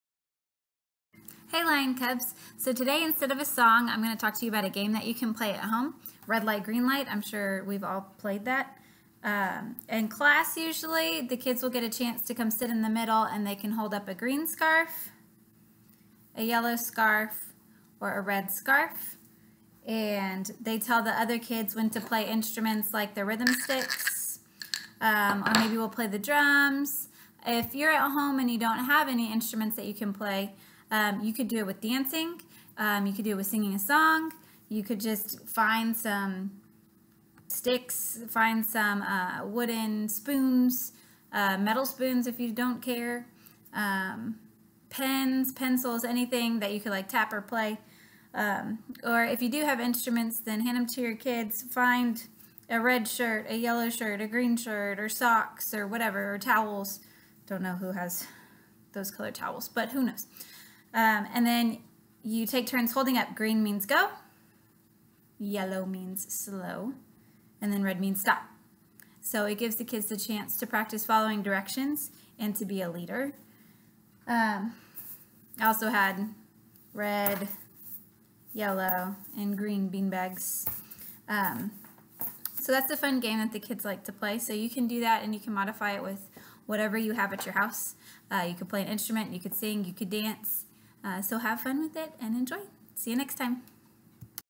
Red Light, Green Light Game
Green - GO
Yellow - SLOW
Red - STOP